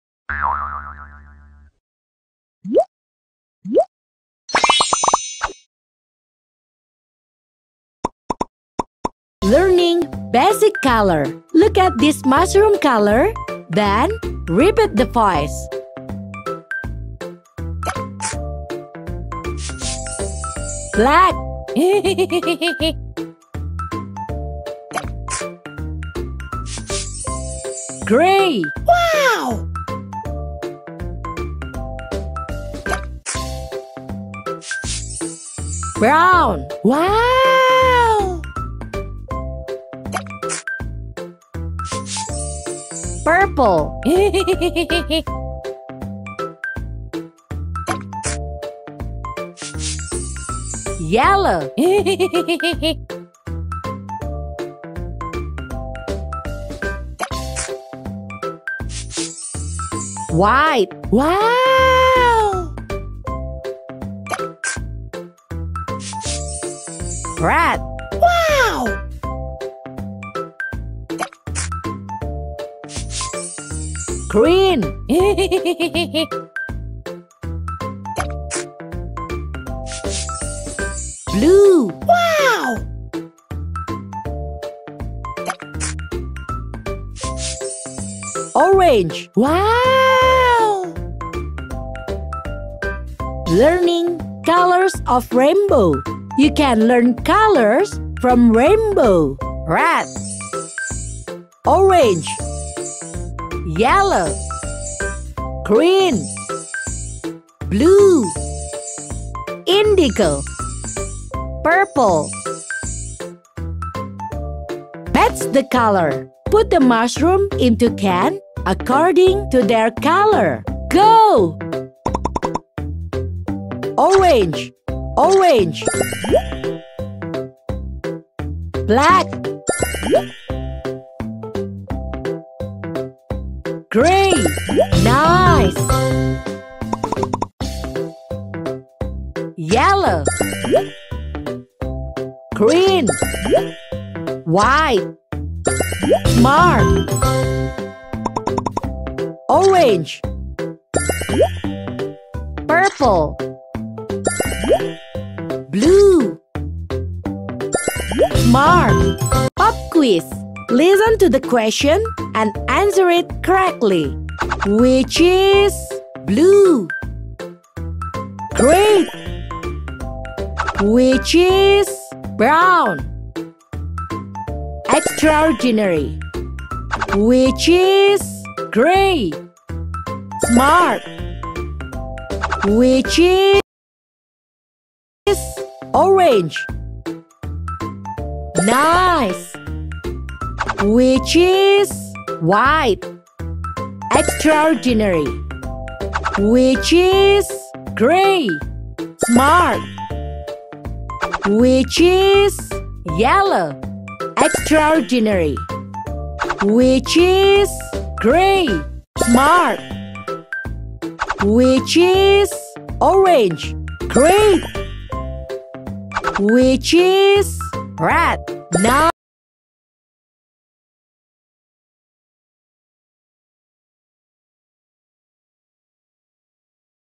LEARN COLORS WITH MUSHROOMS!! YAAY
Let’s have some fun and Learn colors with MUSHROOMS!